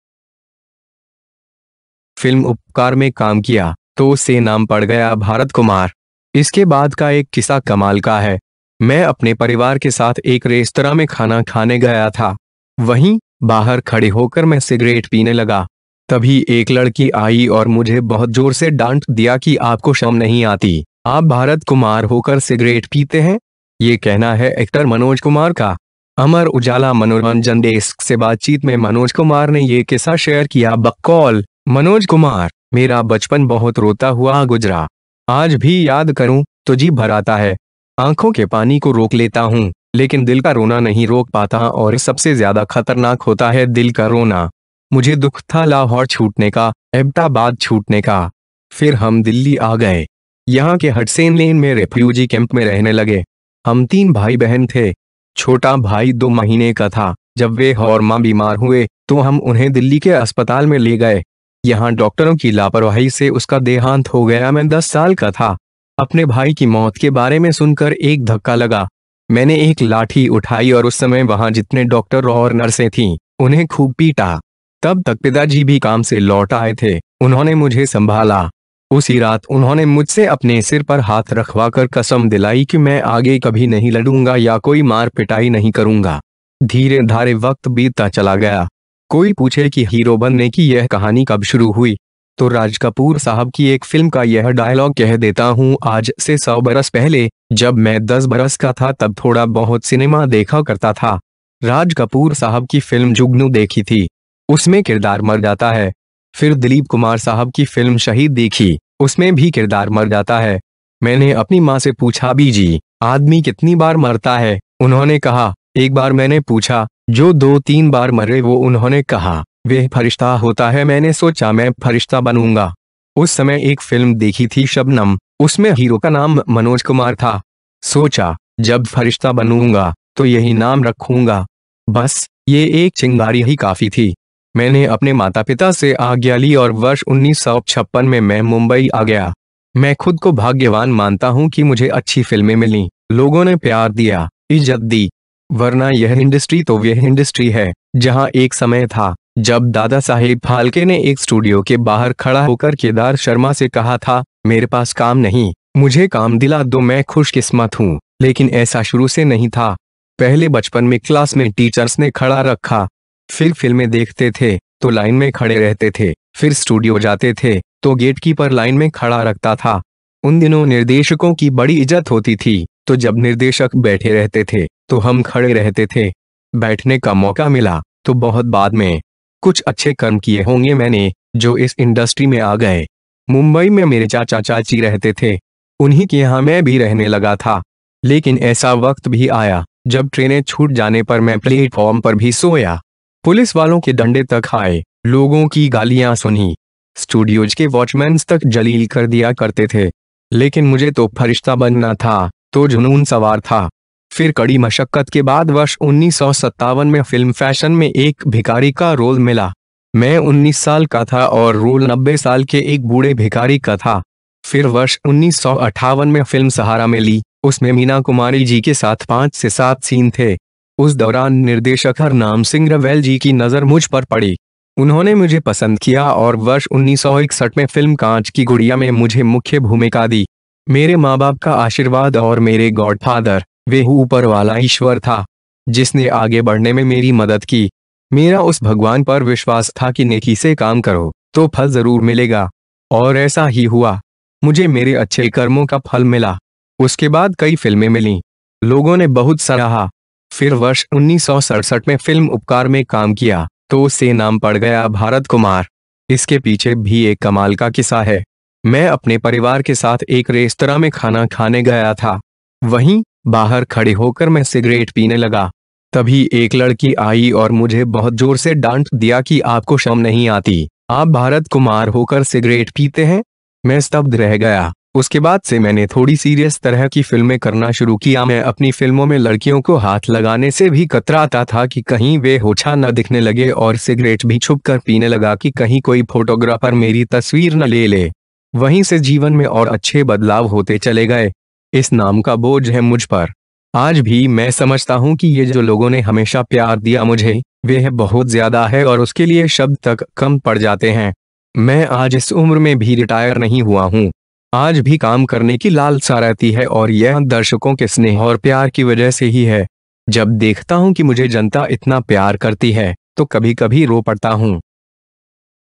एक्टर मनोज कुमार ने खोला 'भारत' होने का दर्द, शेयर किया कमाल का किस्सा
इसके बाद का एक किस्सा कमाल का है। मैं अपने परिवार के साथ एक रेस्तरां में खाना खाने गया था। वहीं, बाहर खड़े होकर मैं सिगरेट पीने लगा। तभी एक लड़की आई और मुझे बहुत जोर से डांट दिया कि आपको शर्म नहीं आती, आप भारत कुमार होकर सिगरेट पीते हैं? ये कहना है एक्टर मनोज कुमार का। अमर उजाला मनोरंजन डेस्क से बातचीत में मनोज कुमार ने ये किस्सा शेयर किया।बकौल, मनोज कुमार, 'मेरा बचपन बहुत रोता हुआ गुजरा। आज भी याद करूं, तो जी भर आता है। आंखों के पानी को रोक लेता हूं, लेकिन दिल का रोना नहीं रोक पाता और सबसे ज्यादा खतरनाक होता है दिल का रोना। मुझे दुख था लाहौर छूटने का, ऐब्टाबाद छूटने का। फिर हम दिल्ली आ गए। यहां के हडसेन लेन में रिफ्यूजी कैंप में रहने लगे। हम तीन भाई-बहन थे। छोटा भाई दो महीने का था, जब वह और मां बीमार हुए, तो हम उन्हें दिल्ली के अस्पताल में ले गए। यहां डॉक्टरों की लापरवाही से उसका देहांत हो गया।मैं दस साल का था। अपने भाई की मौत के बारे में सुनकर एक धक्का लगा। मैंने एक लाठी उठाई और उस समय वहां जितने डॉक्टर और नर्सें थीं, उन्हें खूब पीटा। तब तक पिता जी भी काम से लौट आए थे, उन्होंने मुझे संभाला। उसी रात उन्होंने मुझसे अपने सिर पर हाथ रखवा कर कसम दिलाई कि मैं आगे कभी नहीं लडूंगा या कोई मार-पिटाई नहीं करूंगा। धीरे-धारे वक्त बीतता चला गया। कोई पूछे कि हीरो बनने की यह कहानी कब शुरू हुई, तो राज कपूर साहब की एक फिल्म का यह डायलॉग कह देता हूं- ‘आज से सौ बरस पहले, जब मैं दस बरस का था।’ तब थोड़ा बहुत सिनेमा देखा करता था। राज कपूर साहब की फिल्म 'जुगनू' देखी थी। उसमें किरदार मर जाता है। फिर दिलीप कुमार साहब की फिल्म 'शहीद' देखी, उसमें भी किरदार मर जाता है। मैंने अपनी मां से पूछा-'बीजी, आदमी कितनी बार मरता है?', उन्होंने कहा, ‘एक बार।’ मैंने पूछा, ‘जो दो-तीन बार मरे वो?’ उन्होंने कहा, ‘वह फरिश्ता होता है।’ मैंने सोचा मैं फरिश्ता बनूंगा। उस समय एक फिल्म देखी थी 'शबनम', उसमें हीरो का नाम मनोज कुमार था। सोचा, जब फरिश्ता बनूंगा, तो यही नाम रखूंगा। बस, ये एक चिंगारी ही काफी थी। मैंने अपने माता-पिता से आज्ञा ली और वर्ष 1956 में मैं मुंबई आ गया। मैं खुद को भाग्यवान मानता हूं कि मुझे अच्छी फिल्में मिलीं, लोगों ने प्यार दिया, इज्जत दी। वरना यह इंडस्ट्री तो वह इंडस्ट्री है, जहां एक समय था, जब दादा साहेब फाल्के ने एक स्टूडियो के बाहर खड़ा होकर केदार शर्मा से कहा था, 'मेरे पास काम नहीं, मुझे काम दिला दो।' मैं खुशकिस्मत हूं, लेकिन ऐसा शुरू से नहीं था। पहले बचपन में क्लास में टीचर्स ने खड़ा रखा, फिर फिल्में देखते थे, तो लाइन में खड़े रहते थे, फिर स्टूडियो जाते थे, तो गेटकीपर लाइन में खड़ा रखता था। उन दिनों निर्देशकों की बड़ी इज्जत होती थी, तो जब निर्देशक बैठे रहते थे, तो हम खड़े रहते थे। बैठने का मौका मिला, तो बहुत बाद में। कुछ अच्छे कर्म किए होंगे मैंने, जो इस इंडस्ट्री में आ गए। मुंबई में मेरे चाचा-चाची रहते थे। उन्हीं के यहां मैं भी रहने लगा था। लेकिन ऐसा वक्त भी आया, जब ट्रेने छूट जाने पर मैं प्लेटफॉर्म पर भी सोया। पुलिस वालों के डंडे तक खाए, लोगों की गालियां सुनीं। स्टूडियोज के वॉचमैन्स तक जलील कर दिया करते थे। लेकिन मुझे तो 'फरिश्ता' बनना था, तो जुनून सवार था। फिर कड़ी मशक्कत के बाद वर्ष 1957 में फिल्म 'फैशन' में एक भिखारी का रोल मिला। मैं 19 साल का था और रोल 90 साल के एक बूढ़े भिखारी का था। फिर वर्ष 1958 में फिल्म 'सहारा' मिली, उसमें मीना कुमारी जी के साथ 5-7 सीन थे। उस दौरान निर्देशक हरनाम सिंह रवैल जी की नजर मुझ पर पड़ी। उन्होंने मुझे पसंद किया और वर्ष 1961 में फिल्म 'कांच की गुड़िया' में मुझे मुख्य भूमिका दी। मेरे मां-बाप का आशीर्वाद और मेरे गॉडफादर, वह ऊपर वाला ईश्वर था, जिसने आगे बढ़ने में मेरी मदद की। मेरा उस भगवान पर विश्वास था कि नेकी से काम करो, तो फल जरूर मिलेगा। और ऐसा ही हुआ। मुझे मेरे अच्छे कर्मों का फल मिला। उसके बाद कई फिल्में मिलीं। लोगों ने बहुत सराहा। फिर वर्ष 1967 में फिल्म 'उपकार' में काम किया, तो उससे नाम पड़ गया भारत कुमार। इसके पीछे भी एक कमाल का किस्सा है। मैं अपने परिवार के साथ एक रेस्तरां में खाना खाने गया था। वहीं, बाहर खड़े होकर मैं सिगरेट पीने लगा। तभी एक लड़की आई और मुझे बहुत जोर से डांट दिया कि आपको शर्म नहीं आती, आप भारत कुमार होकर सिगरेट पीते हैं? मैं स्तब्ध रह गया, उसके बाद से मैंने थोड़ी सीरियस तरह की फिल्में करना शुरू कियामैं अपनी फिल्मों में लड़कियों को हाथ लगाने से भी कतराता था कि कहीं वह ओछा न दिखने लगे और सिगरेट भी छुपकर पीने लगा कि कहीं कोई फोटोग्राफर मेरी तस्वीर न ले ले। वहीं से जीवन में और अच्छे बदलाव होते चले गए। इस नाम का बोझ है मुझ पर। आज भी मैं समझता हूं कि ये जो लोगों ने हमेशा प्यार दिया मुझे, वह बहुत ज्यादा है और उसके लिए शब्द तक कम पड़ जाते हैं। मैं आज इस उम्र में भी रिटायर नहीं हुआ हूं। आज भी काम करने की लालसा रहती है और यह दर्शकों के स्नेह और प्यार की वजह से ही है। जब देखता हूं कि मुझे जनता इतना प्यार करती है, तो कभी-कभी रो पड़ता हूं।